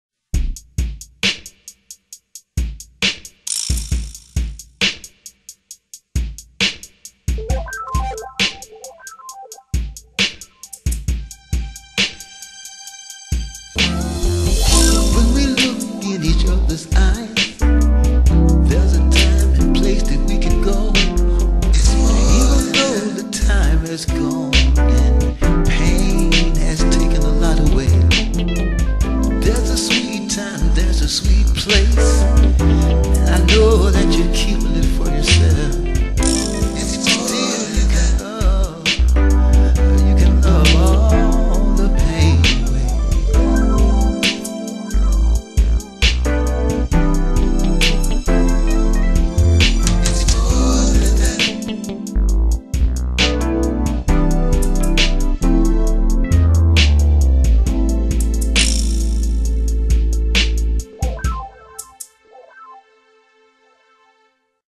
Aspiring Artist- How Does This Track Sound?
This is a friend of mine who is aspiring to promote and produce his music. Listen to this short demo and provide him some feedback on his track. He makes his own music. Thanks for your comments.